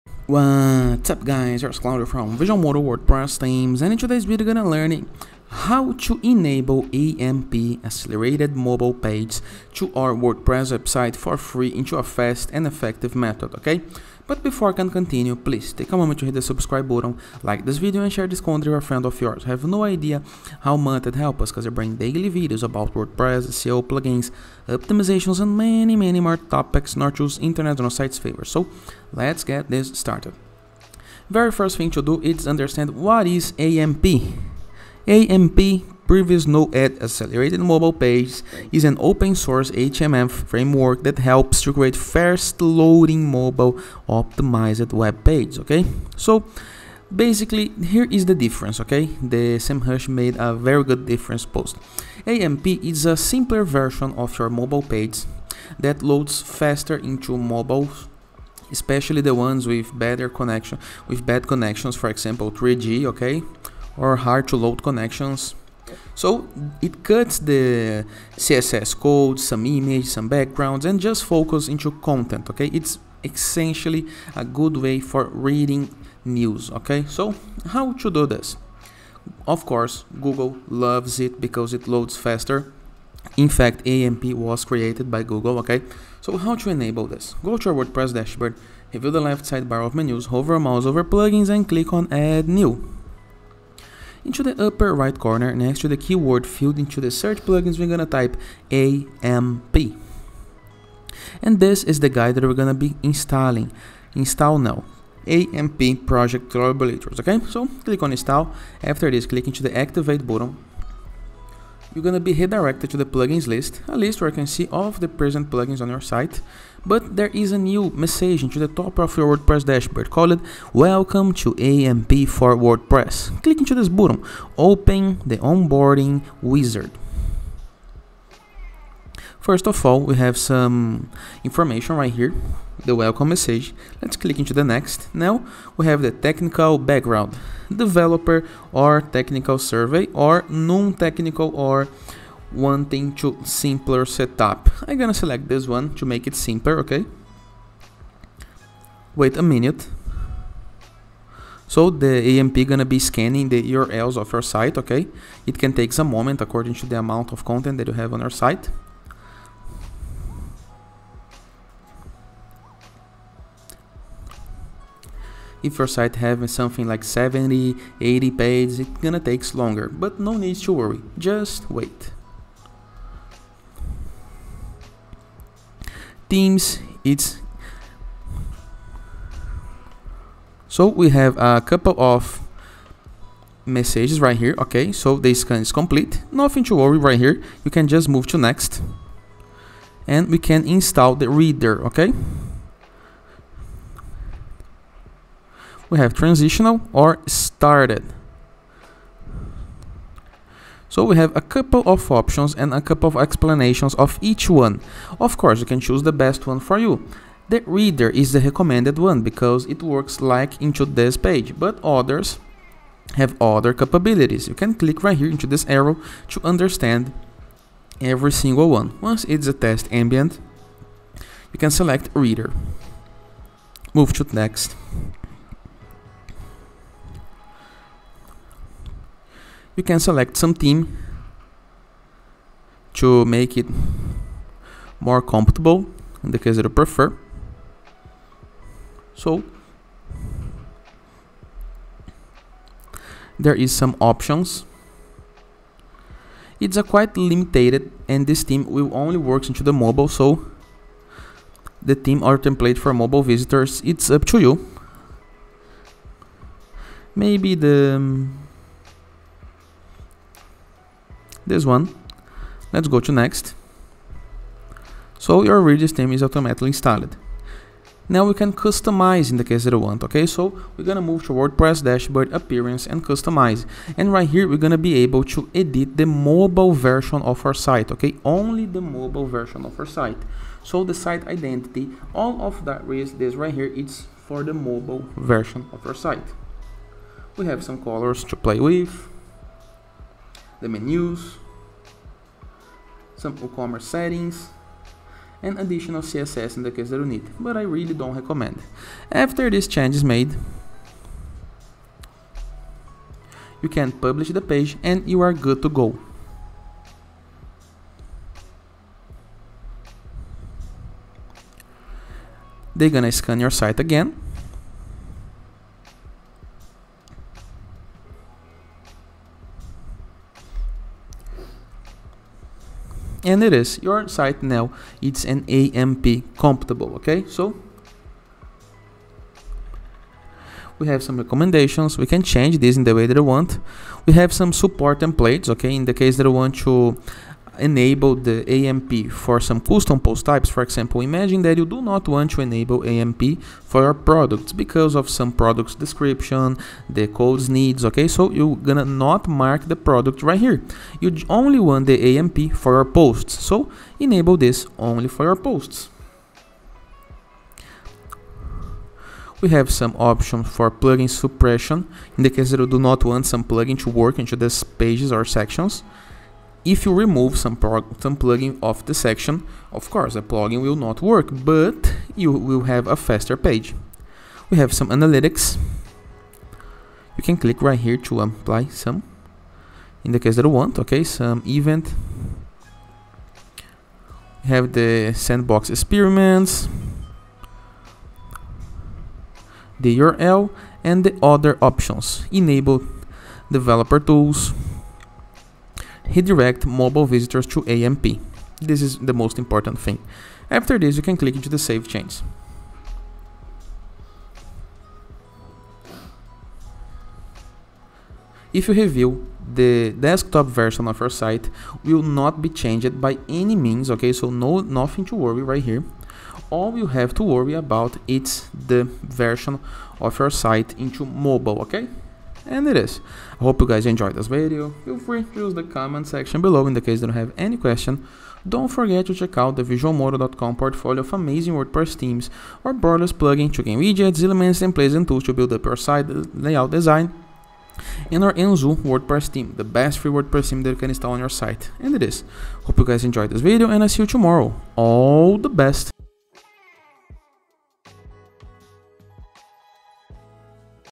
📱 How to Enable AMP Accelerated Mobile Pages in Your WordPress Website For Free? Setup Tutorial 🚀
🚀 Unlock Lightning-Fast Mobile Pages! 🌩 Learn How to Enable AMP on Your WordPress Site for FREE! [Detailed Tutorial]

Are you ready to skyrocket your WordPress site's mobile performance? Join us as we dive into the world of Accelerated Mobile Pages (AMP)! In this comprehensive, step-by-step tutorial, we'll guide you through the entire process of enabling AMP on your WordPress website - absolutely free!

Discover the secrets to seamless AMP integration and watch your site's speed soar, enhancing user experience, boosting engagement, and potentially improving your search rankings. No coding skills? No problem! We've got you covered with simple, actionable instructions.

🔎 What You'll Master:
The strategic benefits of AMP for your mobile audience and SEO.
A foolproof method for selecting and installing the best AMP plugin.
Customizing your AMP pages to maintain brand consistency.
Techniques to ensure your AMP pages are fully optimized and error-free.
Pro tips to enhance AMP page engagement and visibility in search results.

If you have any questions, drop them in the comments below - I'm here to help!

Join our exclusive SEO and WordPress tips mailing list [insert subscription link] for insights, updates, and cutting-edge strategies to keep your website ahead of the curve.